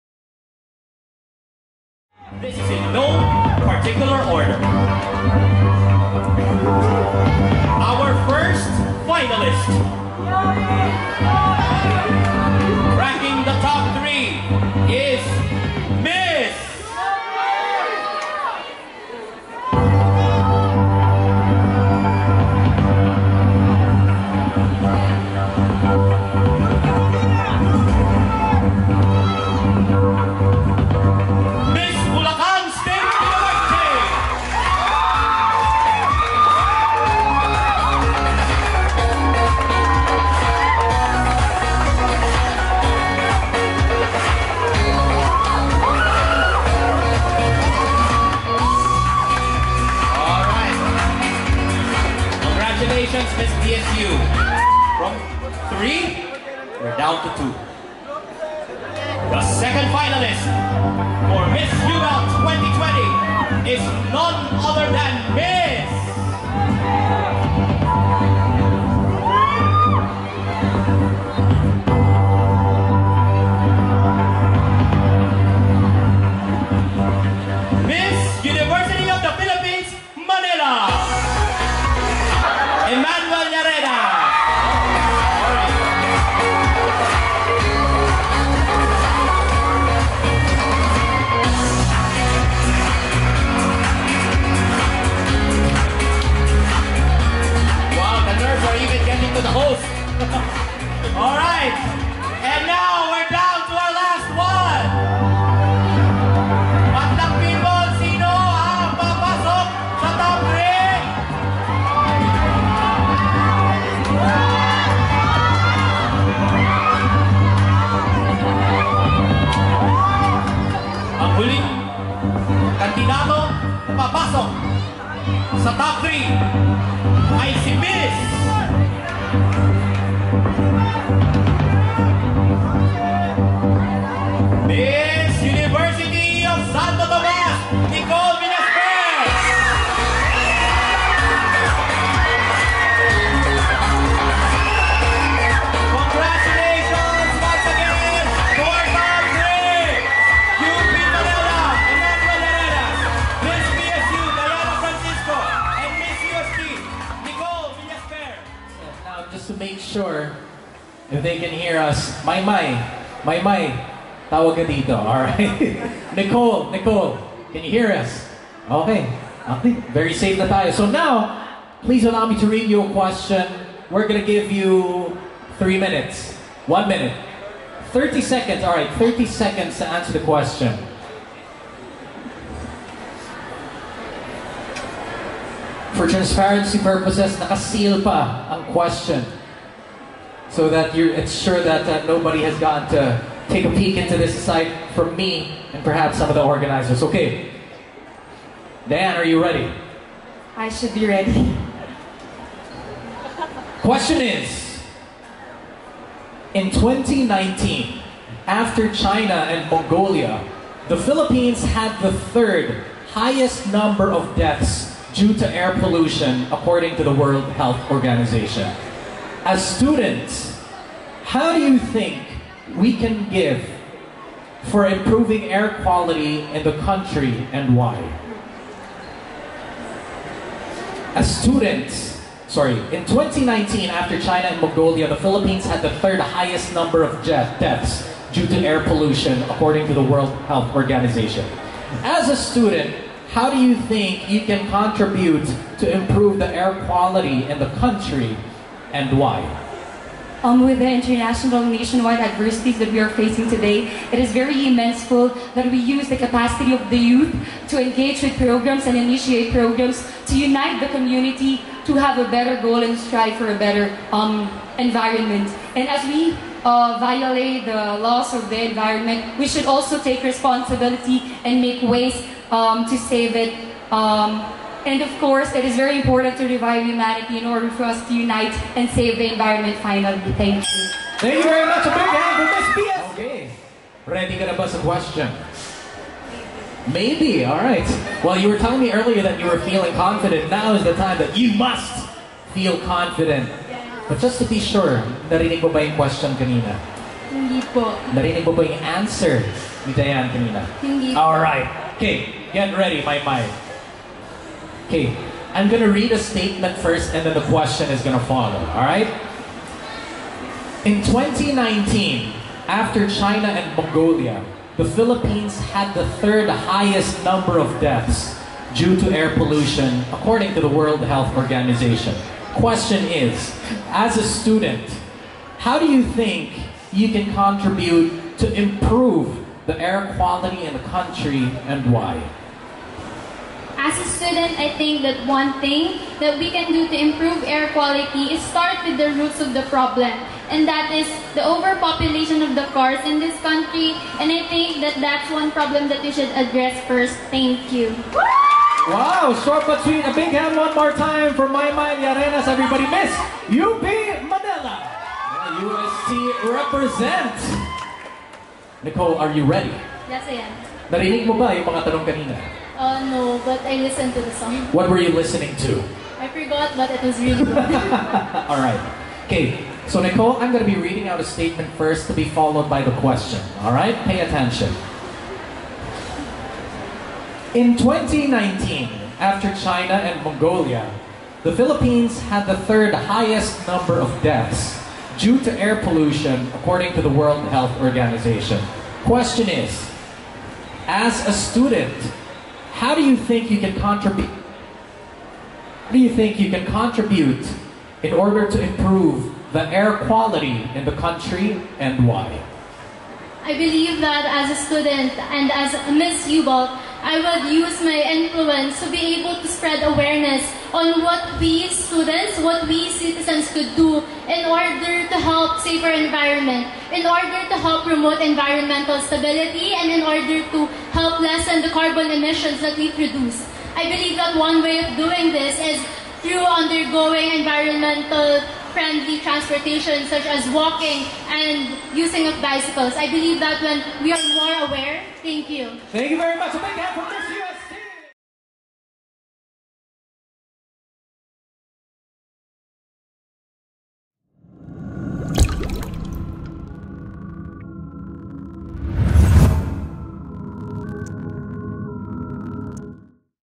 OFFICIAL: MISS-UBELT 2020, The TOP 3 and Final Q&A.
An annual intercollegiate beauty pageant participated by students from various colleges & universities in the Philippines. We do not claim ownership of all music materials used in the actual event and in the video.
This event is presented by, Isetann Dept., Store, Inc., Pond's, Cream Silk, Juicy Cologne.